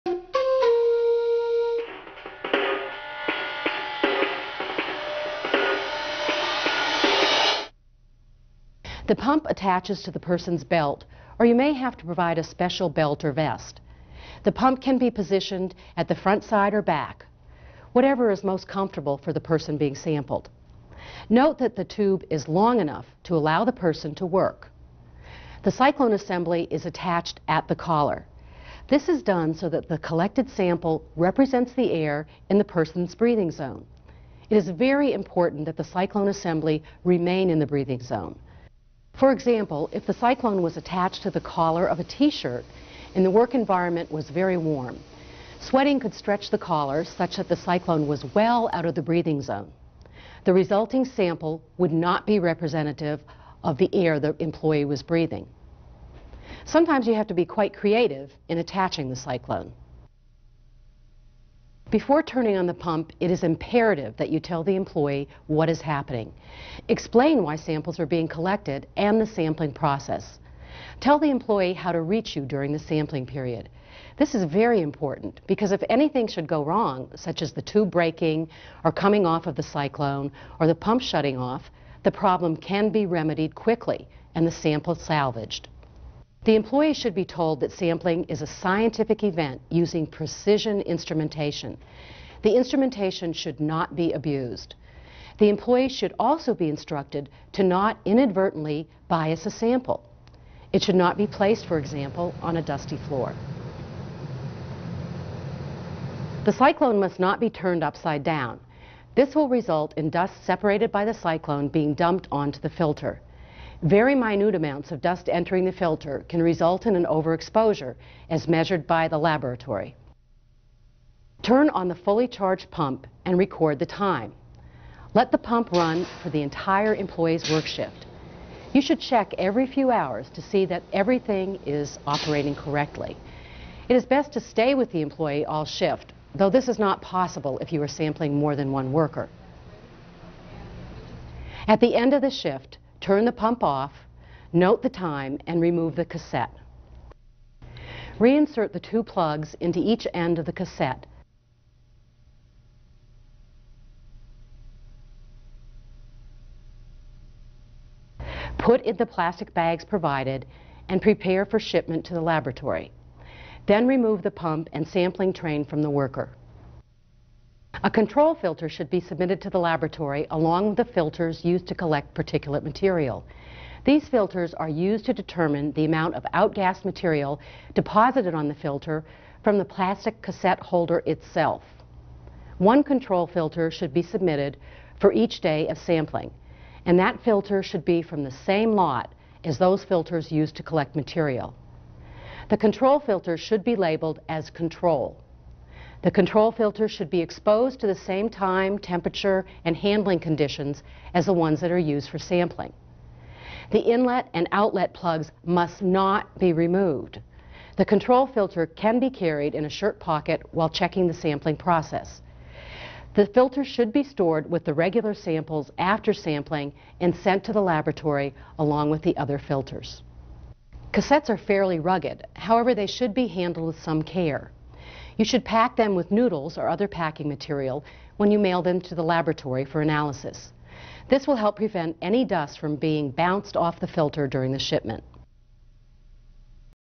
Obtaining a Dust Sample
Mine Safety and Health Administration

This is Part 3 of 4 Parts
DVD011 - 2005 - Respirable Dust Sampling